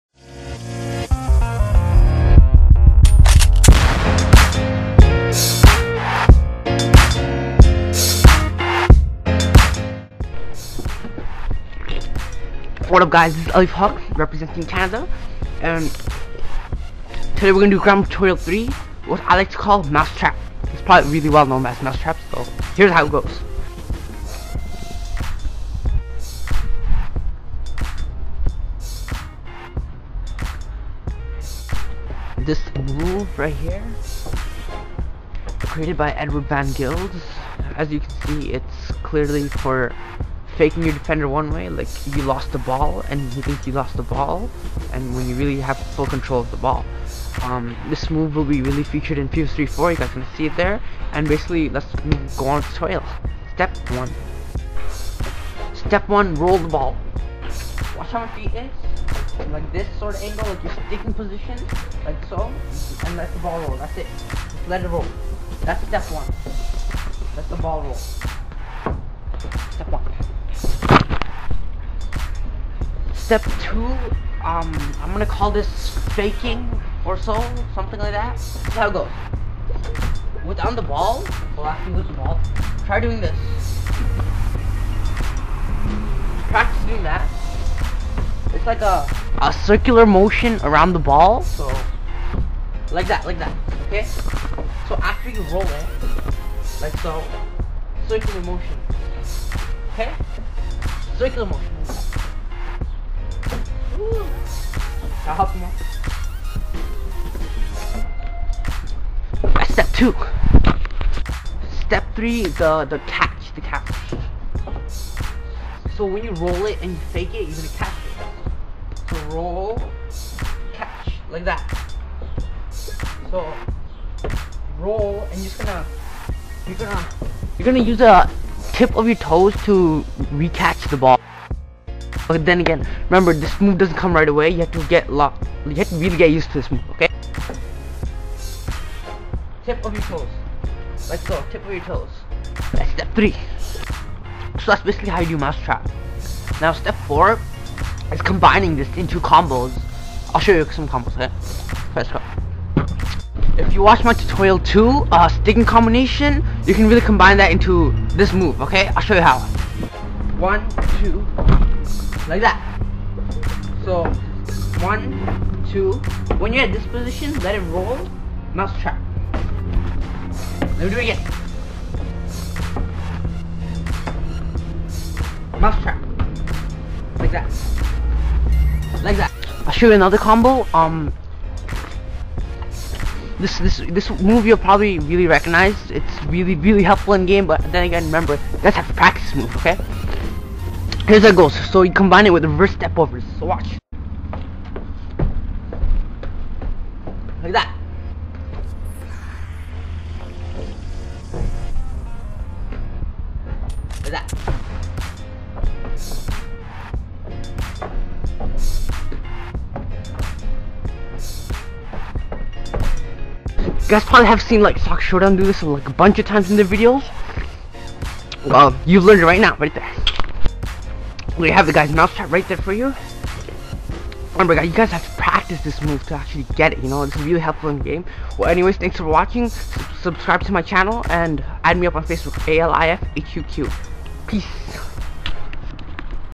Groundmove Tutorial #3
What up guys :D

 Alif Huq with groundmove tutorial 3 : The Mouse Trap

Really usefull move but requires ALOT of practise ;)

Thanks :D